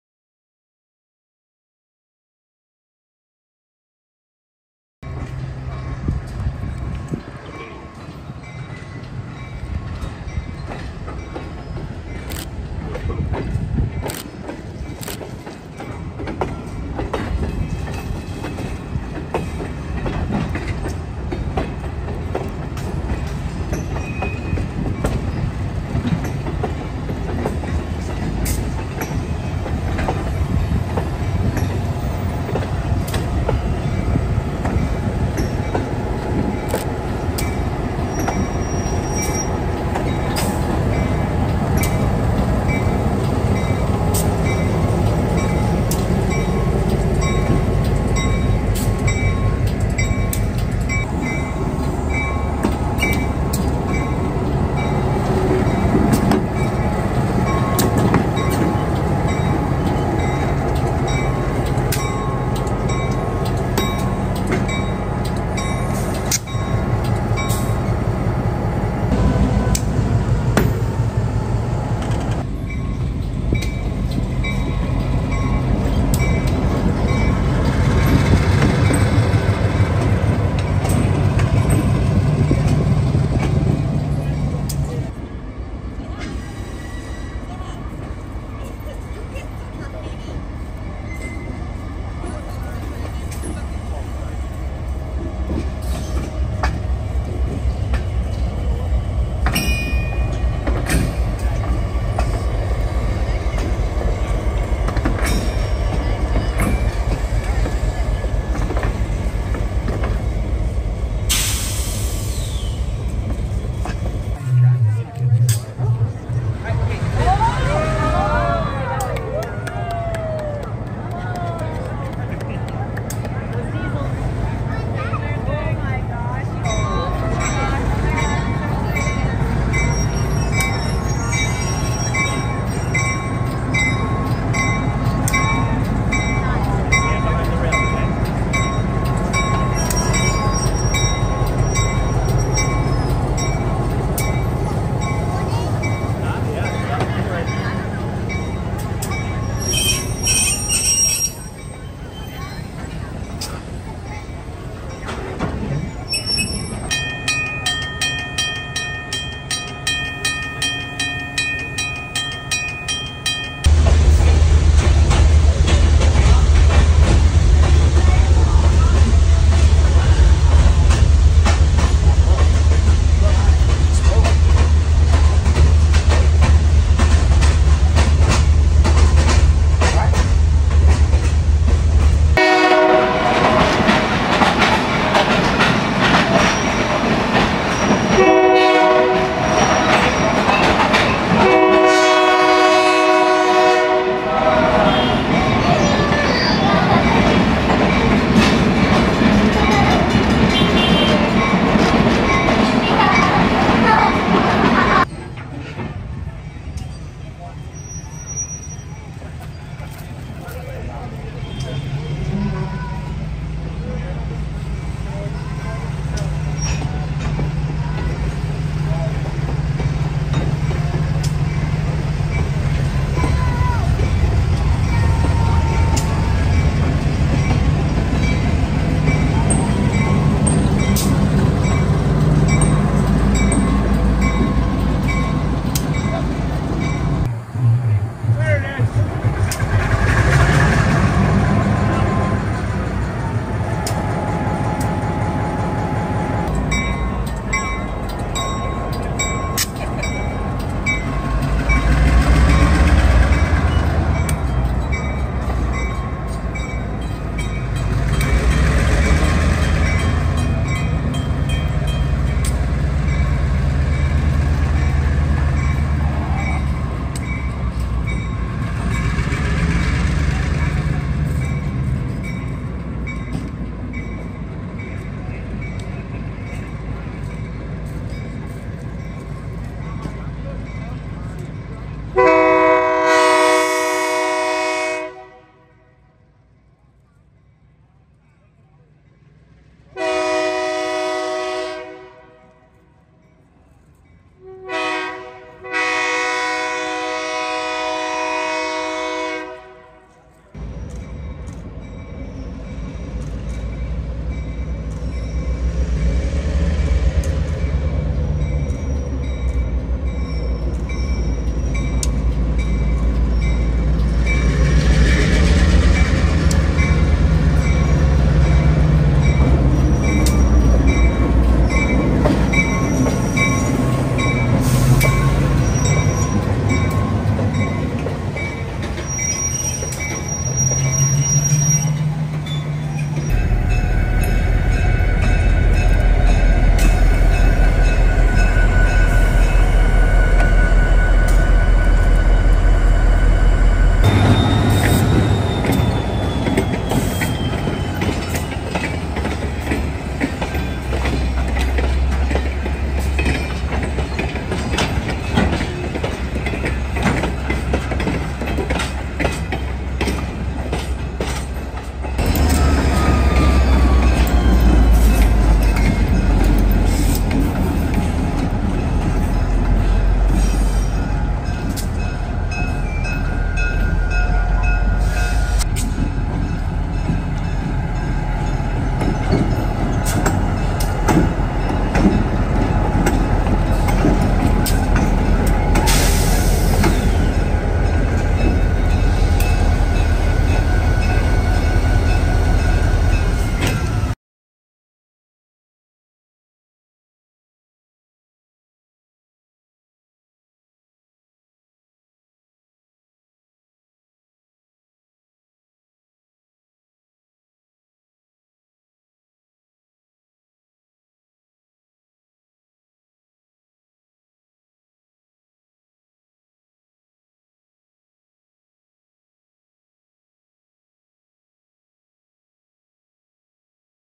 Whippany Railway Museum 5/19/24
Sunday, May 19th, 2024, marked the 175th anniversary that the Central Railroad of New Jersey (CNJ or Jersey Central Lines) was founded. Whippany Railway Museum hosted an event to celebrate this historic day. NJT 4109 was there visiting the museum it came in on M&E train MO91 Thursday night. The day before all the engines plus 4109 were outside the M&E shops in Morristown so I stopped by and got a picture. On the day of the event I went to Whippany, I rode the train pulled by one of M&E's "new" GP15-1s ( 24 is an ex-Missuiori Pacific, EX Union Pacific/ Union Pacific Yard (UPY) GP15-1). It was nice seeing it and riding behind it plus we went westbound toward Morristown which is rare for Whippany to do which was a nice change of pace from the normal eastbound excursions.